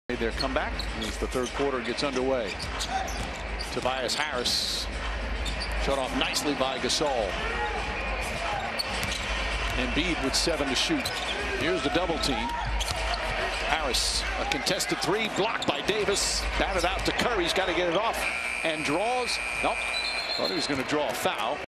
LAKERS defense forces 24 seconds violation
LAKERS defense forces 24 seconds violation by 76ers.